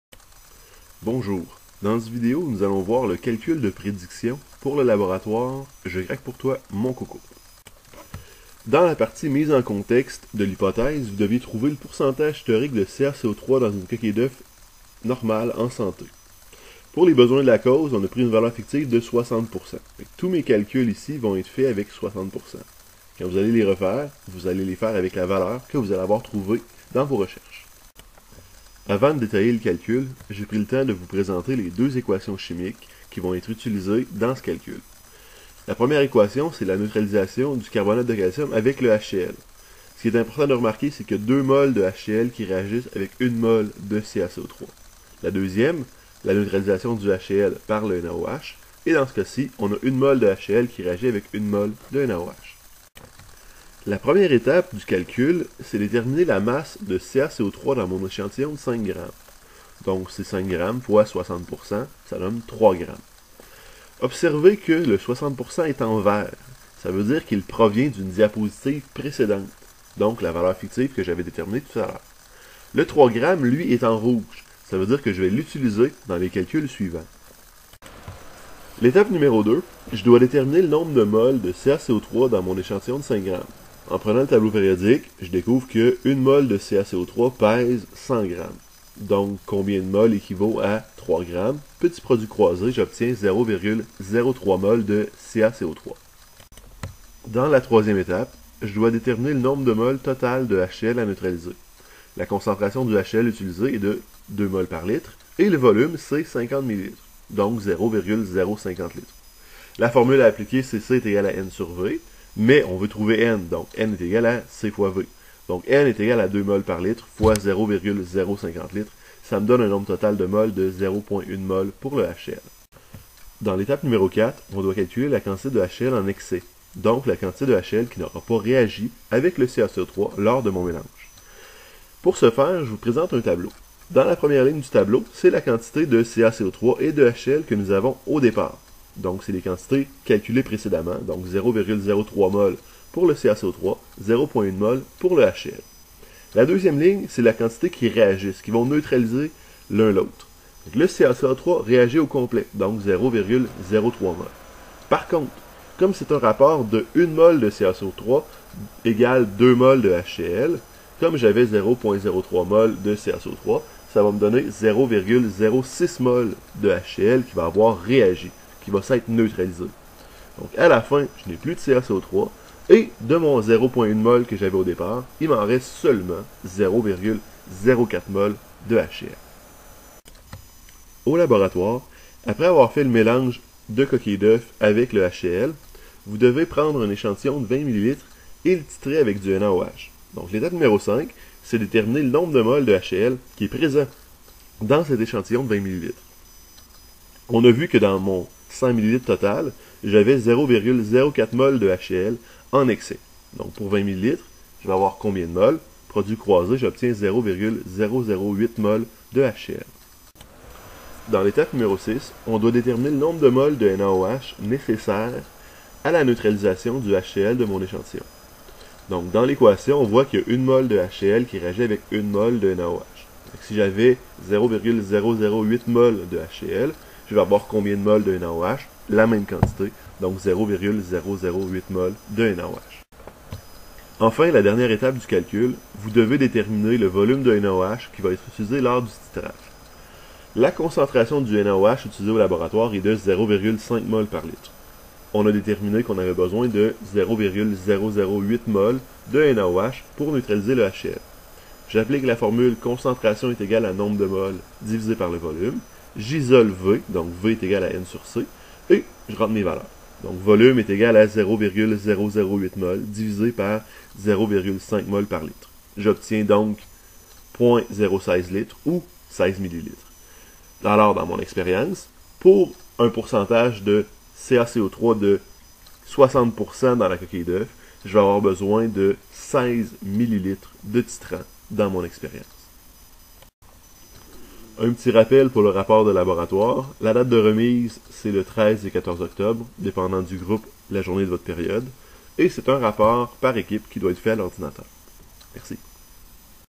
Calcul de prédictions pour le laboratoire : Je craque pour toi mon coco
Explications pour le calcul de prédictions pour la section "Prédictions" du rapport de laboratoire "Je craque pour toi mon coco".